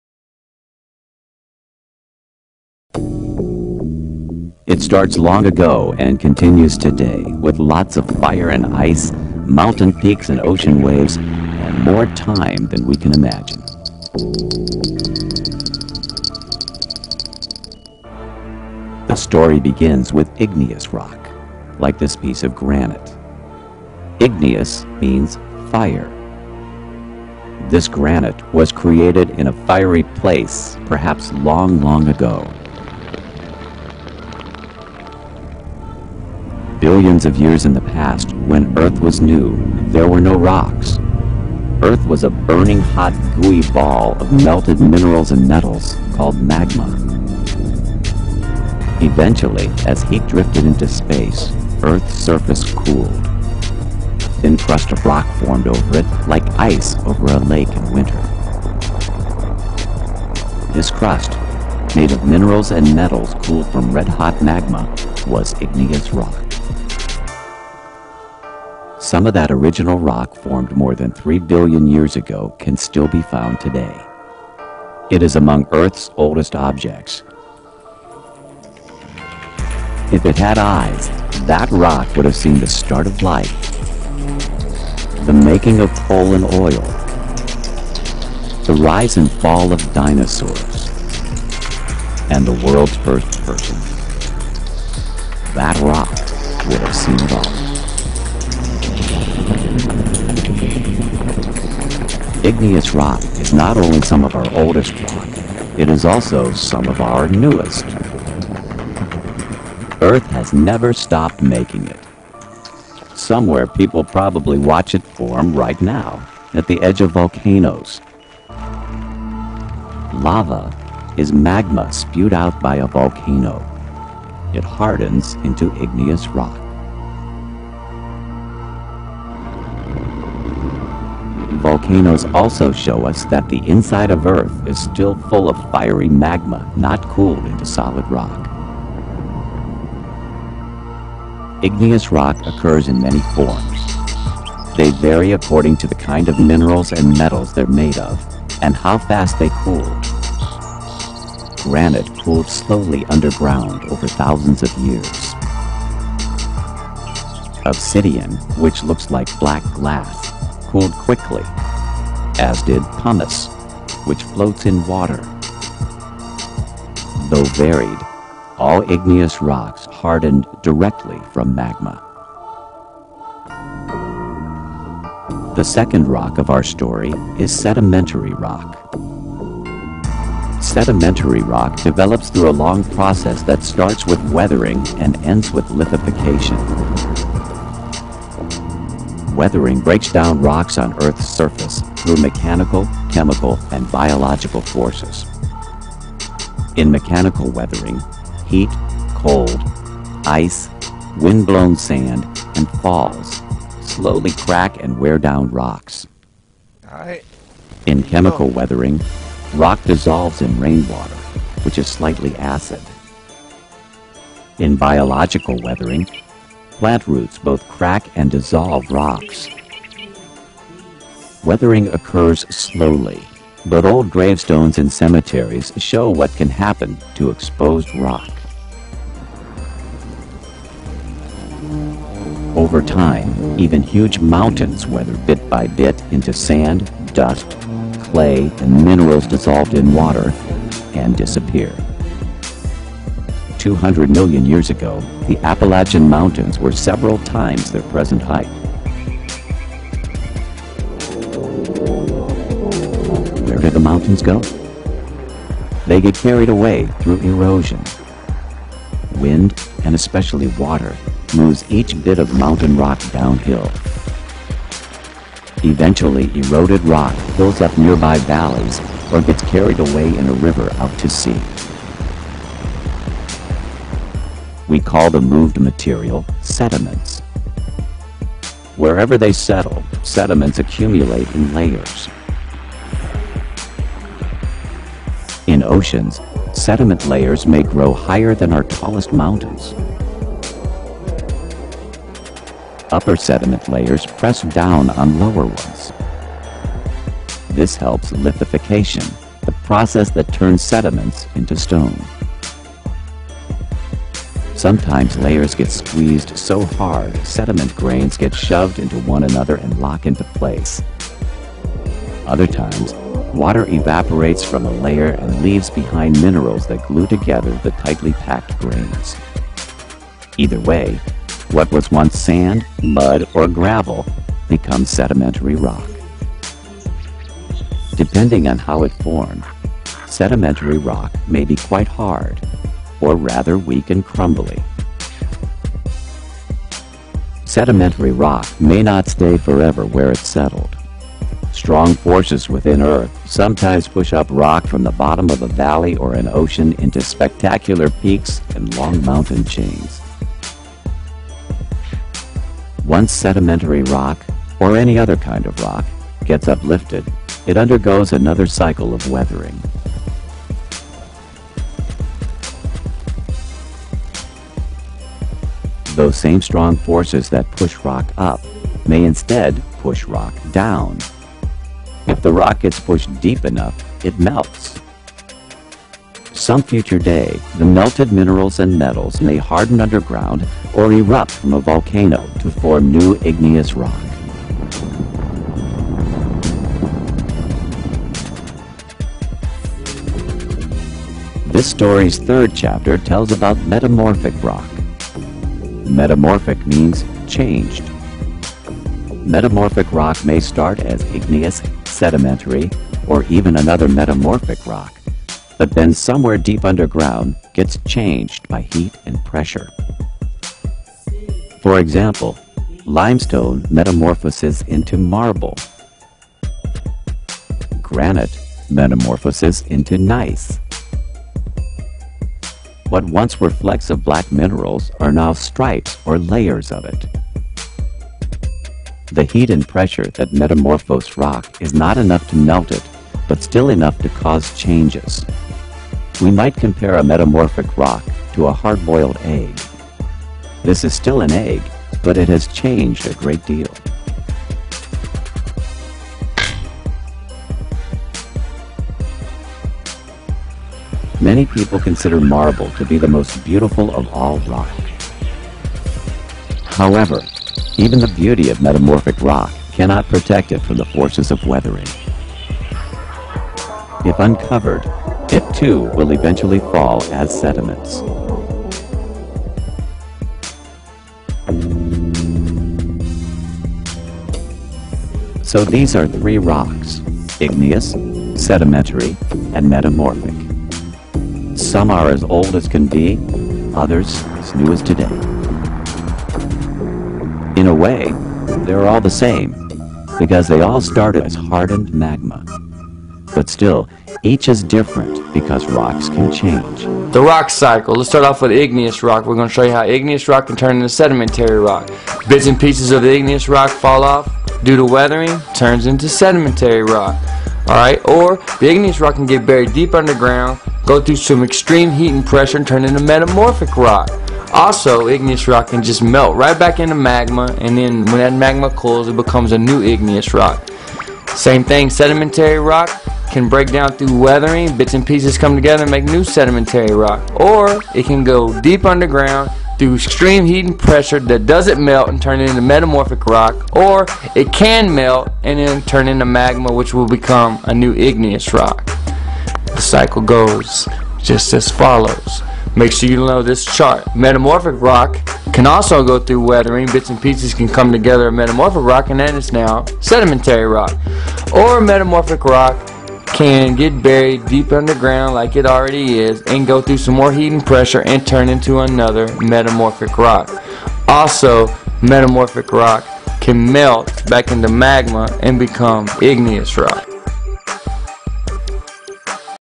Three Main Rocks and the Rock Cycle
The rock cycle graphically displays the processes involved in the formation
of the three types of rocks: igneous rocks form from melting and
cooling, metamorphic rocks by heat and pressure, and sedimentary rocks
by weathering, transportation, deposition and lithification. Each type may
be formed from either one of the other two rock types or from the same
rock type.
findmarble.net